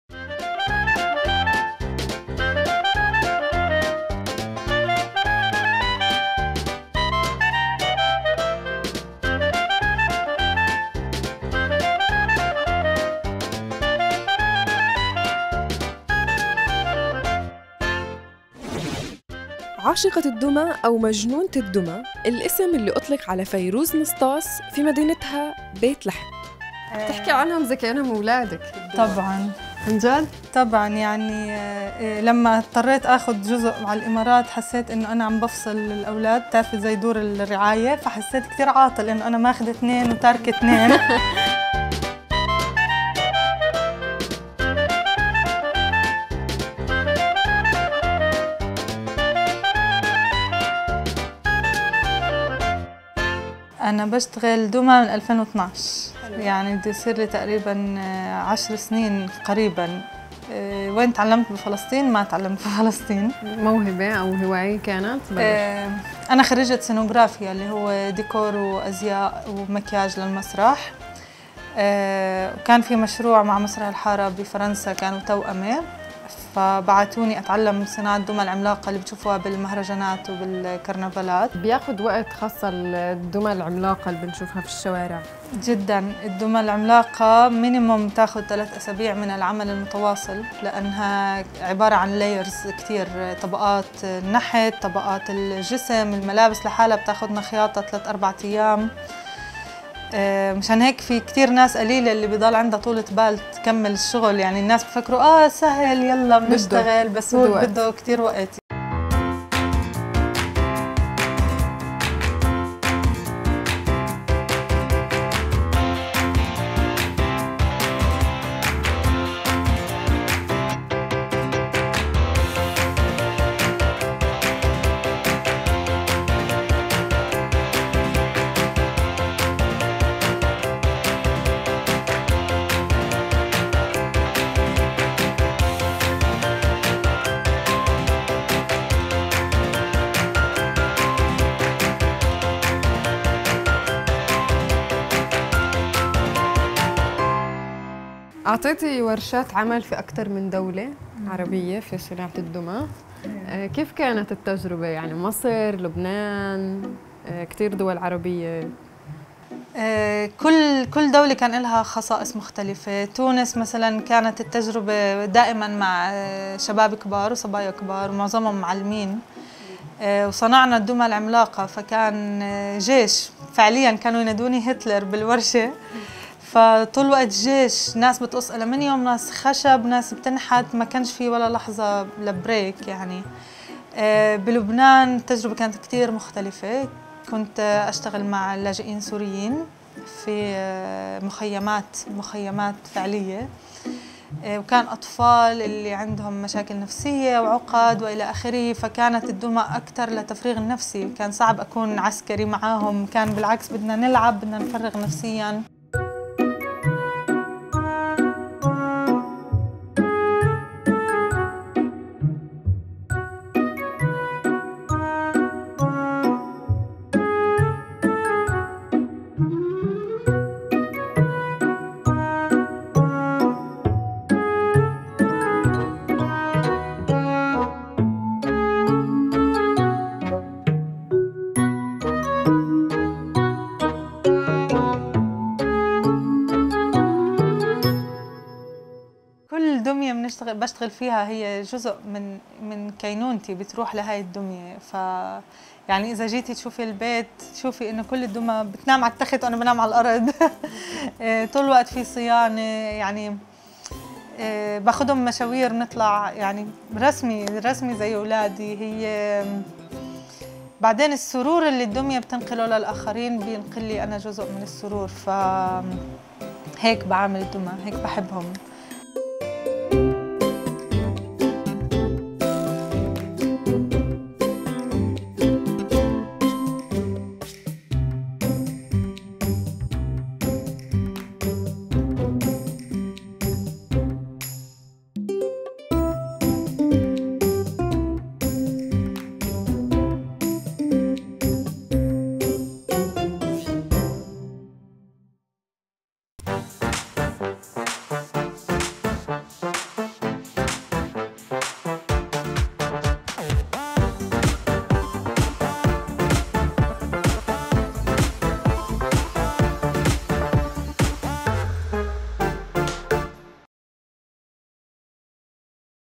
مجنونة الدمى الفلسطينية "فيروز نسطاس" صانعة ومدربة معتمدة
مجنونة الدمى الفلسطينية "فيروز نسطاس" صانعة ومدربة معتمدة في وطنها ودول عربية اخرى